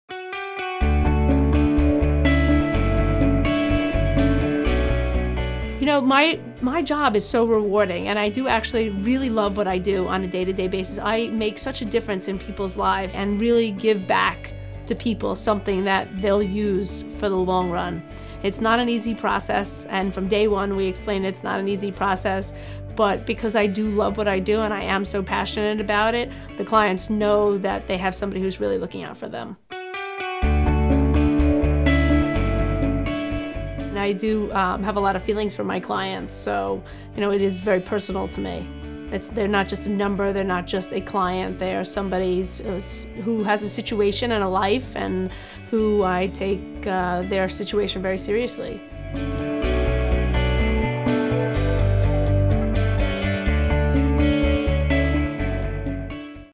Leslie Tayne, Esq. loves what she does!
Leslie H. Tayne, Esq. of Law Offices of Leslie H. Tayne, P.C. discusses how special and important her debt resolution practice is to her and how she feels she makes it a different experience for clients by truly loving what she does.  She is so passionate about her work that she feels that she makes a difference in each and every client's lives by helping them settle their debts and get back on their feet on the road to financial freedom once and for all!